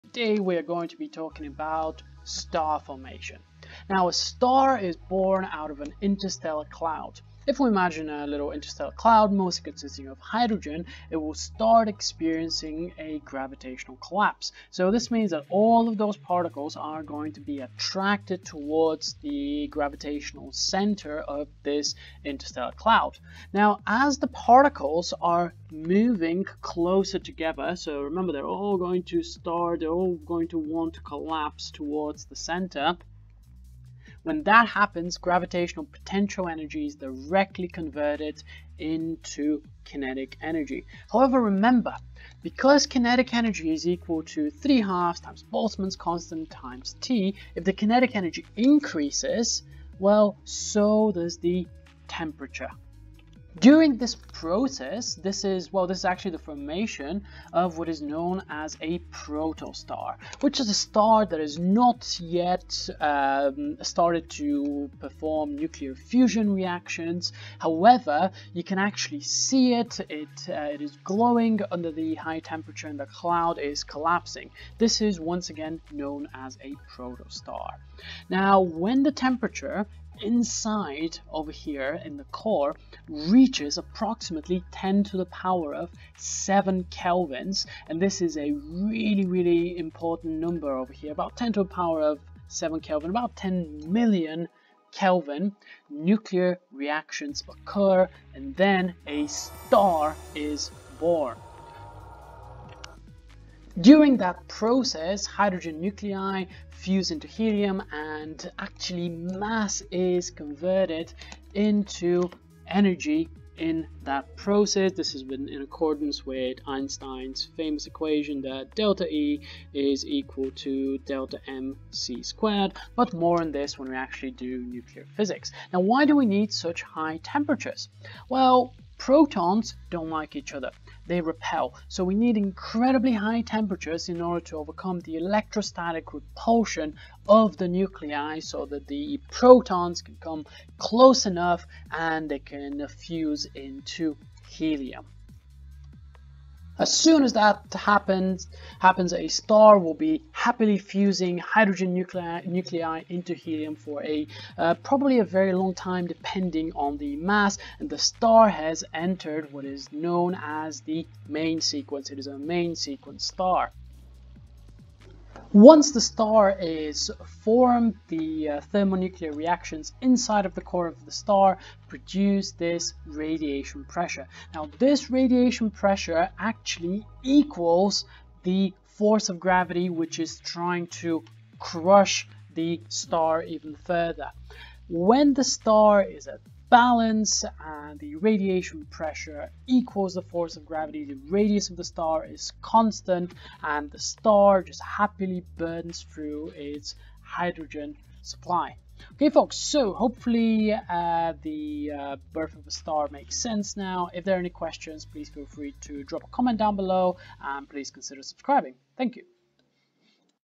A Level Physics: Star Formation
Chapters:
00:00 Intro
00:10 The collapse of an interstellar cloud
01:10 Proto star
01:43 Thermonuclear Reactions
02:57 Gravity and Radiation Pressure